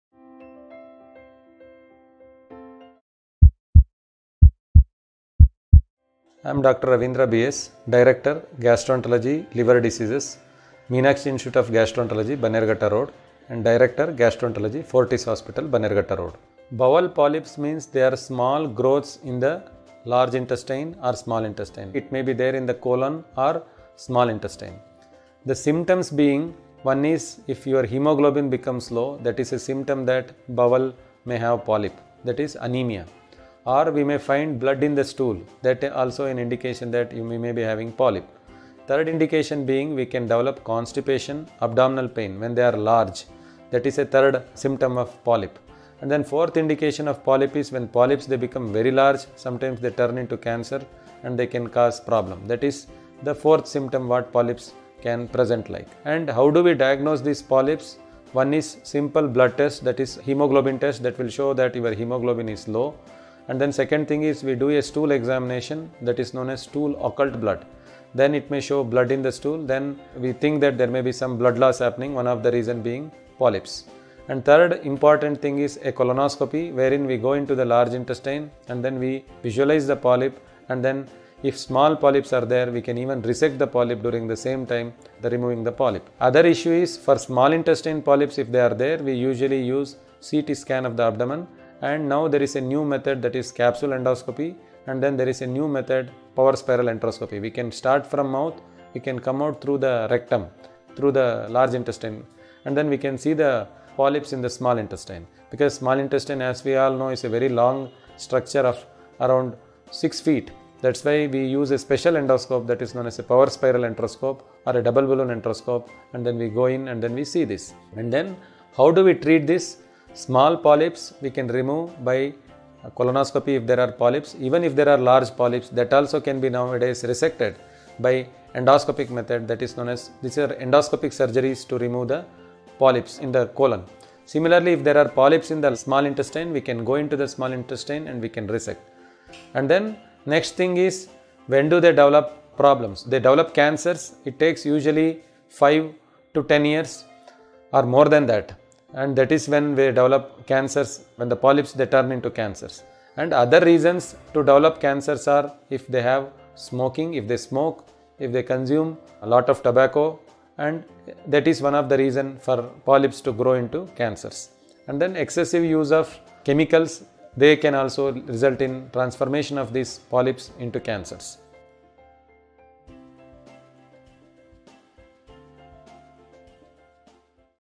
BOWEL POLYPS | How long does it take for a POLYP TO TURN INTO CANCER?-Dr.Ravindra BS|Doctors' Circle
Dr Ravindra BS | Appointment Booking No. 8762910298
Consultant Gastroenterologist & Hepatologist | Meenakshi Gastro Liver Care,Bengaluru
Bowel polyps means they are  small growths in the large intestine or the small intestine. It might be in the colon or the small intestines.   The symptoms being if your hemoglobin becomes  low. It may be that your bowel may have polyp, that is anemia or we may find blood in the stool. So that is also an indication that there may be Bowel polyp.